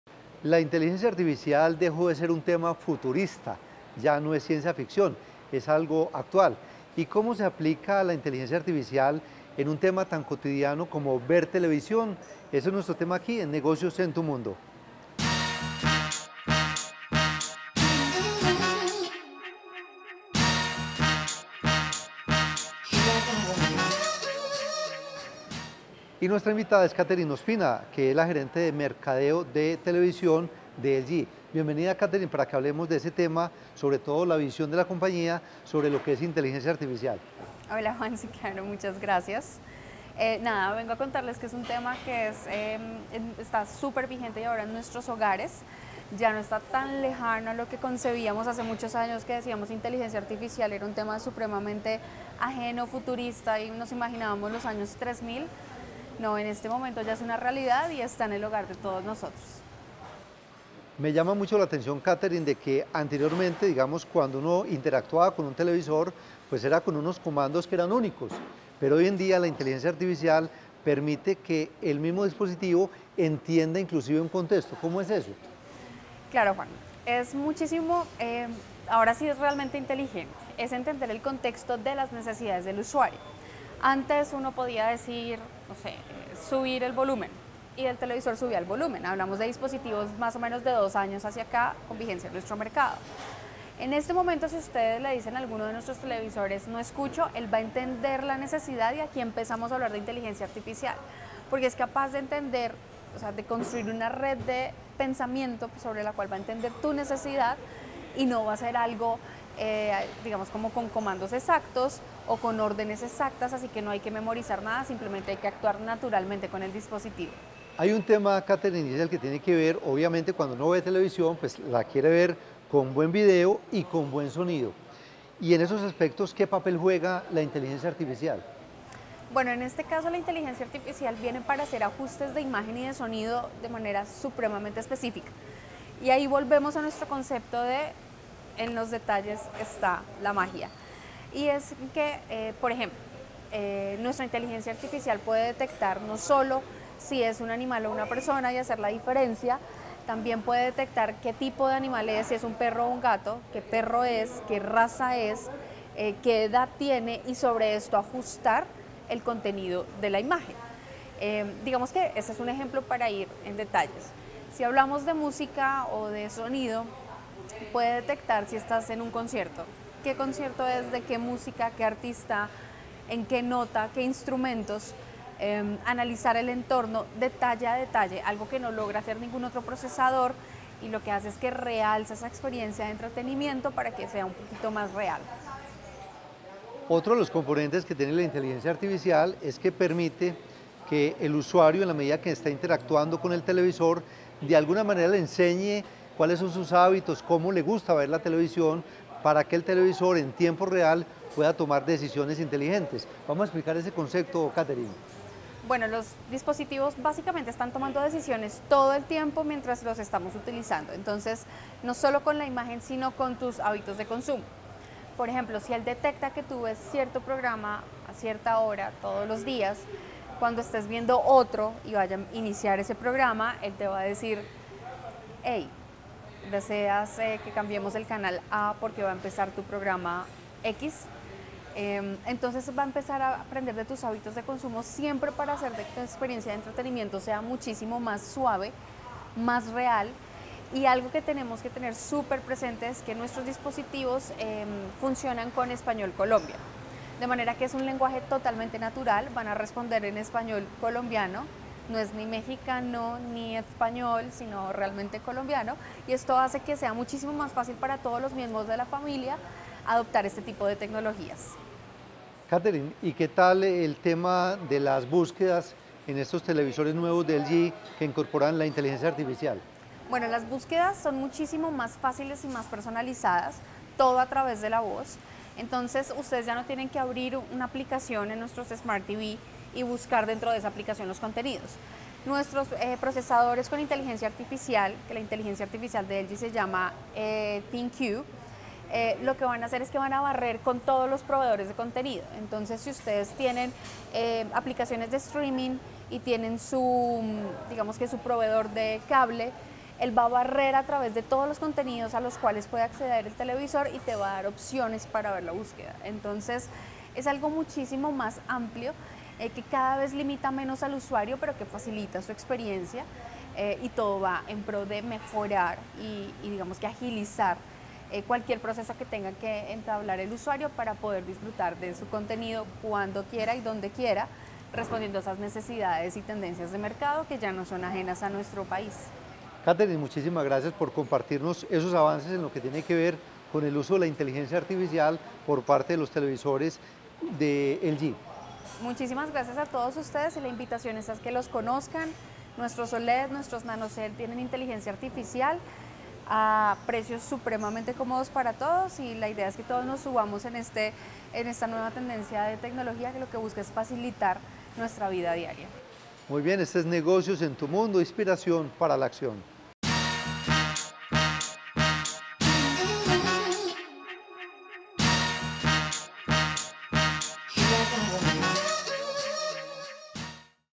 LG ThinQ AI: Televisores inteligentes para televidentes exigentes - [Negocios en Tm] ®
Así funciona la inteligencia artificial en los televisores

Mejorar la imagen y el sonido son apenas el comienzo de lo que la inteligencia artificial puede hacer en un equipo de televisión. Lo más sorprendente es la capacidad de un televisor de entender lo que alguien le dice y generar una acción. Por ejemplo, si alguien menciona: "no estoy escuchando", el equipo de inmediato sube el volumen. Así la inteligencia artificial elimina los comandos de voz únicos y los reemplaza por expresiones naturales de una persona, que son comprendidos por un equipo de televisión. Catherin Ospina, Gerente de mercadeo de televisión de LG, nos explica el poder la inteligencia artificial.

Negociosentm